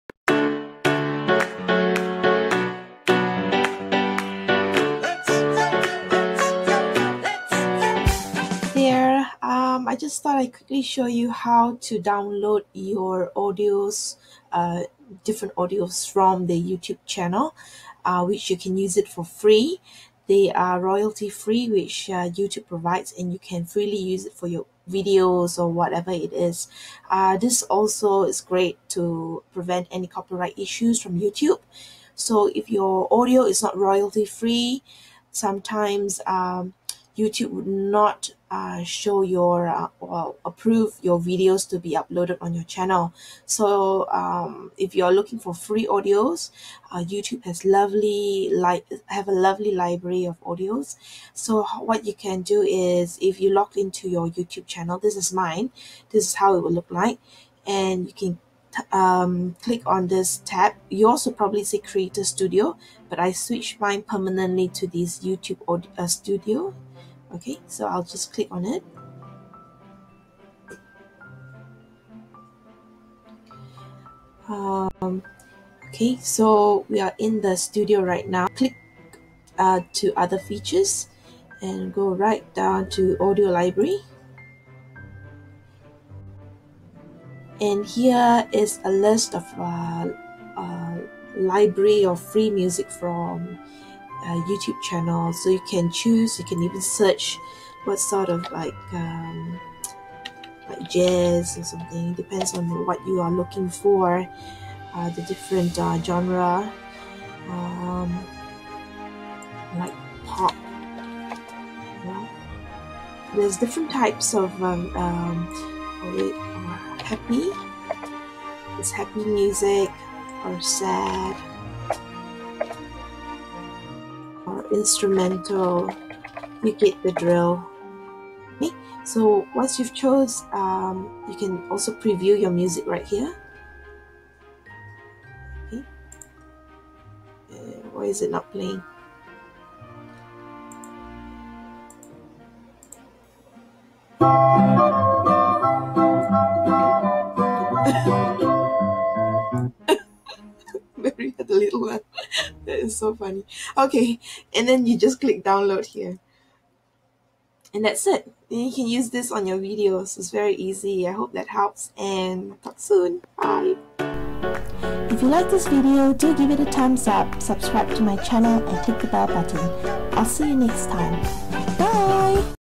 How to Download Your Free Audio Tracks from YouTube Studio (Beta)
Want to edit your videos breezily? Try the latest Filmora

YouTube has a lovely library of audio tracks which you can freely use for your videos! Try it today and hope you'll enjoy this very simple tutorial today!

It's so easy to edit videos and audio in Filmora. And for those who wants to take their video to the next level, you can check it out below: